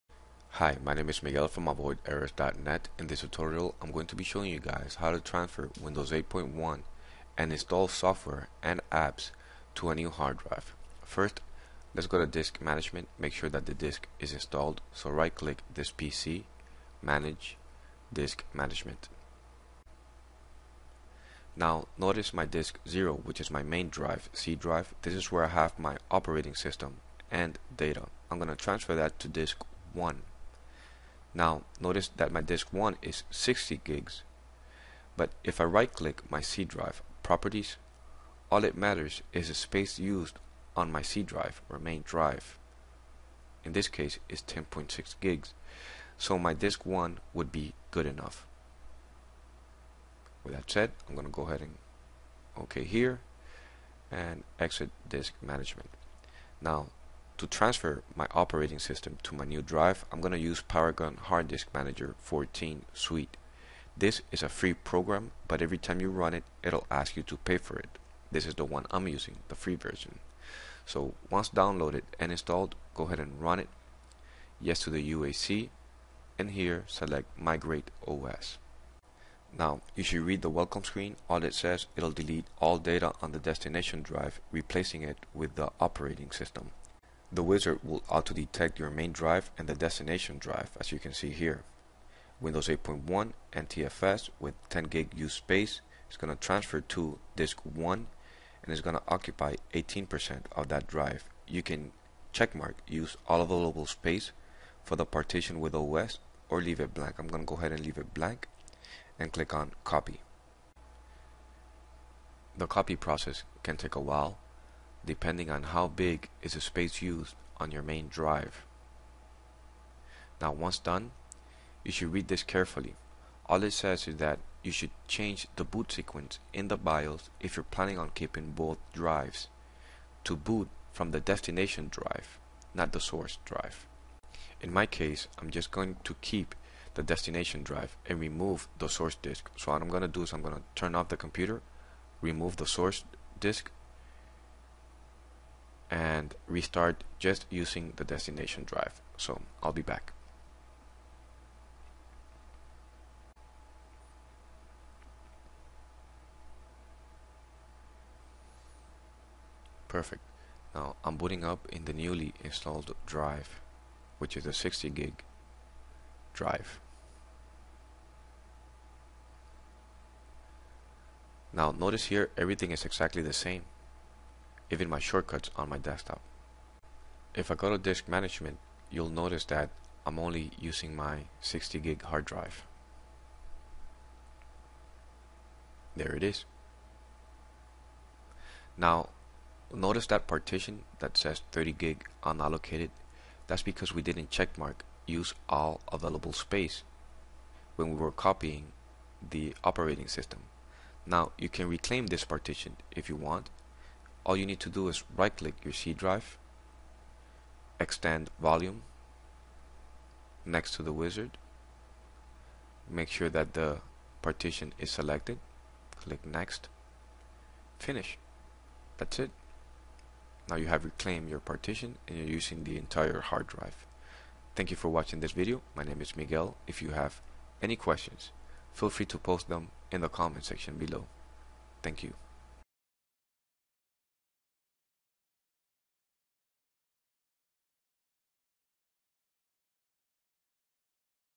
Transfer Windows 8.1 and Installed Data to a New Hard Drive
To transfer Windows 8.1 to a new HDD will eventually save a hell lot of time, you don't have to install all programs and configure settings from scratch.